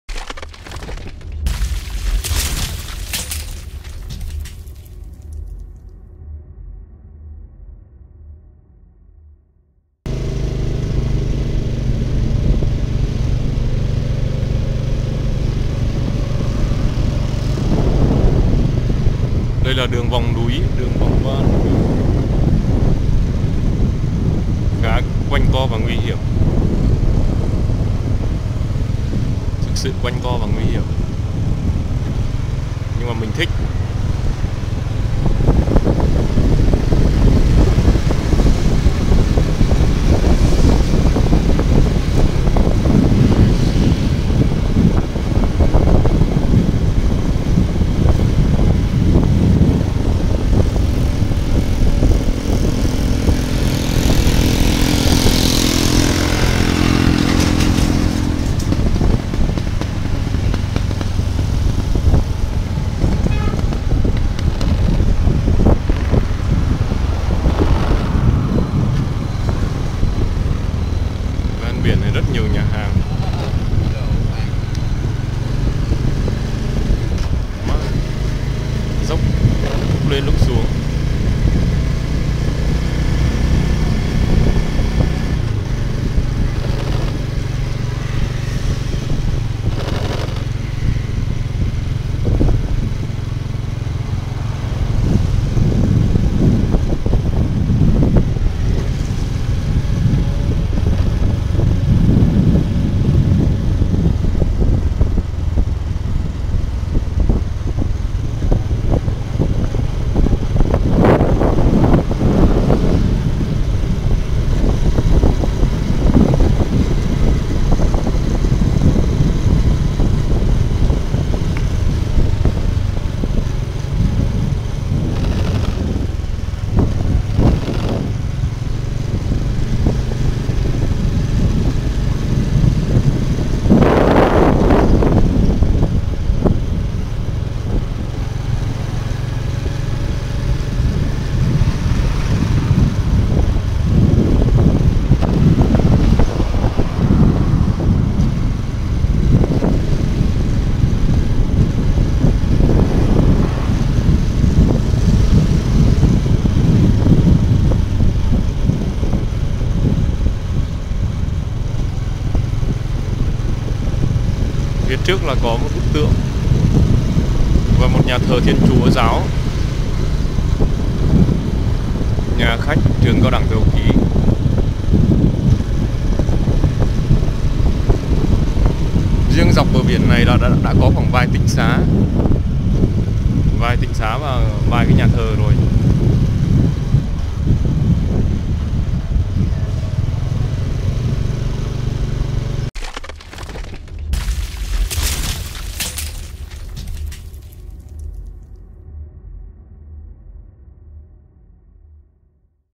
Khám Phá Vũng Tàu: Đường Ven Biển Bãi Dâu Đẹp Bậc Nhất Thành Phố Biển
Khám phá Vũng Tàu với con đường ven biển Bãi Dâu đẹp bậc nhất thành phố du lịch. Bãi Dâu nằm ở phía Tây núi Lớn và nằm ở phía bắc của thành phố Vũng Tàu.
Đi từ bờ biển bãi Trước, men theo đường Trần Phú ven biển, đi qua di tích Bạch Dinh chừng 3 km thì bạn sẽ tới bãi Dâu. Trước khi có tên gọi bãi Dâu, nơi đây được gọi là Vũng Mây do nơi đây có nhiều mây rừng. Ngày nay, cây mây và dâu rừng không còn nhưng người ta thường nhớ đến bãi Dâu vì đây là một bãi biển kín gió, nhiều ghềnh đá kỳ thú và thơ mộng. Hai đầu bãi có nhiều mỏm đá lớn nhô ra ngoài biển, sau lưng bãi là địa hình lòng chảo được cây cối um tùm bao bọc tựa vào triền núi Lớn.